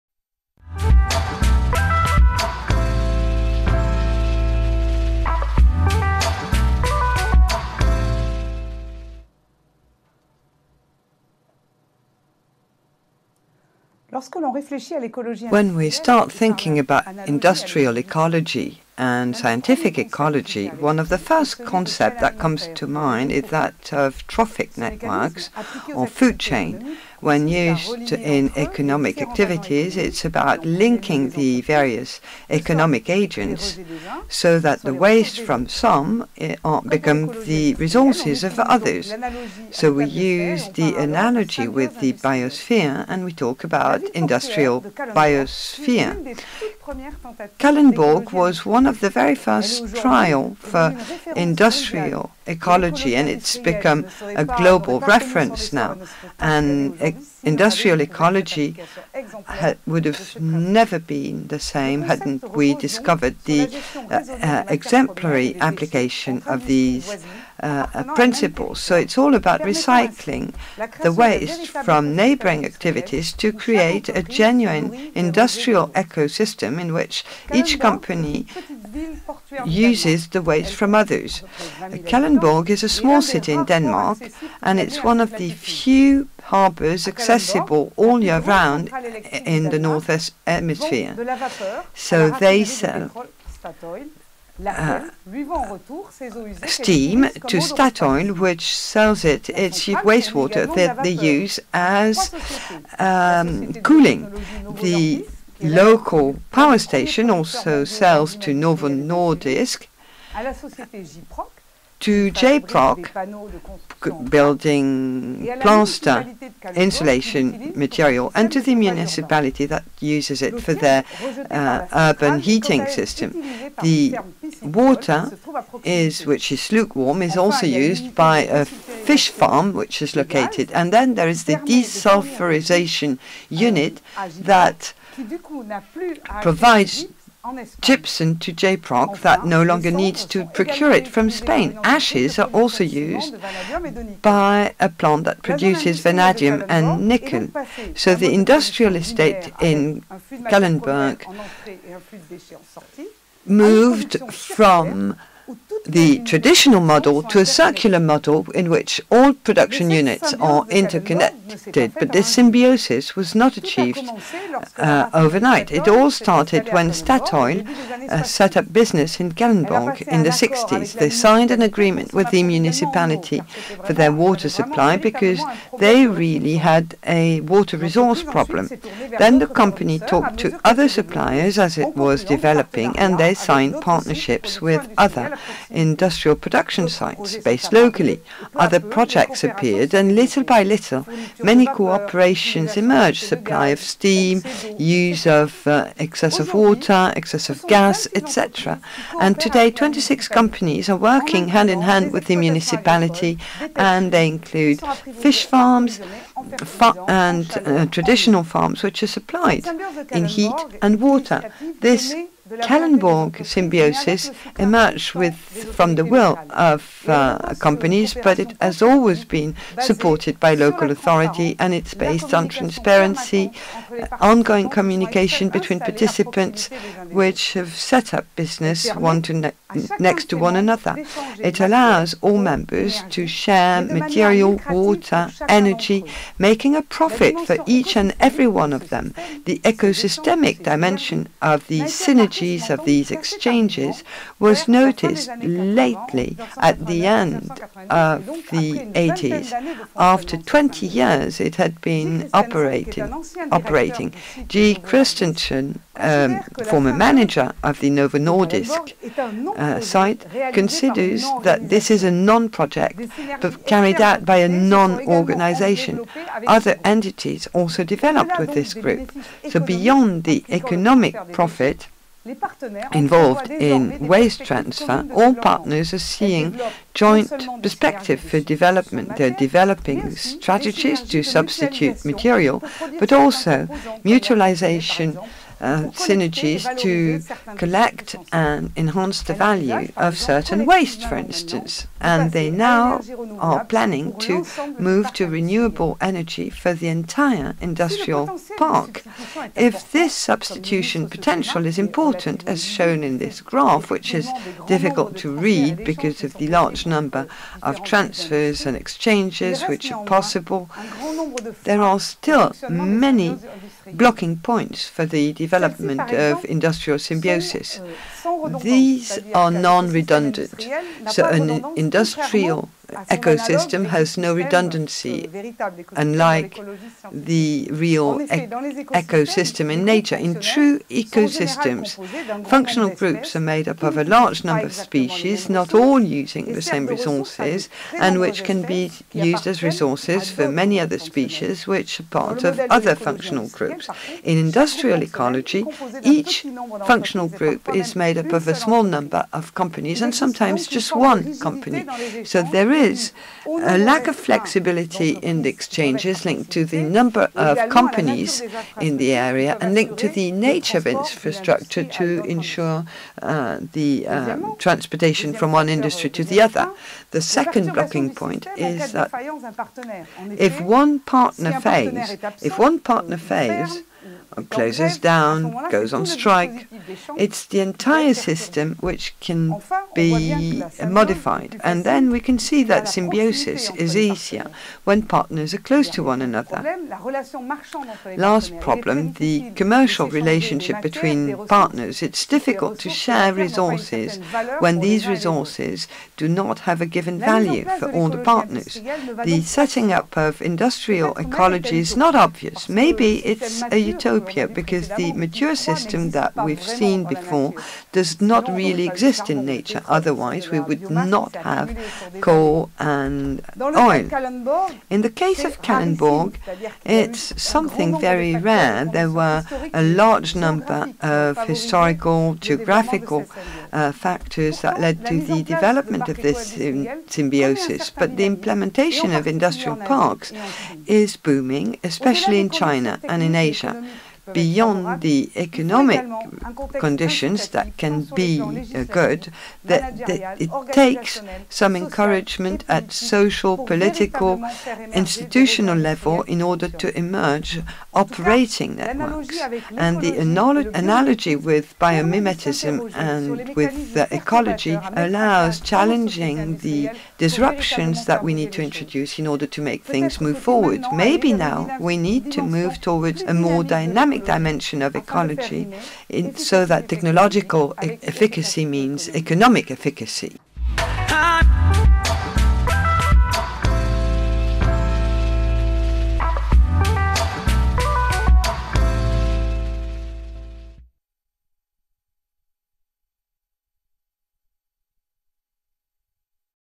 Industrial symbioses and eco-industrial parks : the Kalundborg case
Frédérique Vincent, professor at Mines-ParisTech, presents an example of the implementation of an industrial ecosystem. It is the symbiosis of Kalundborg, located in Denmark.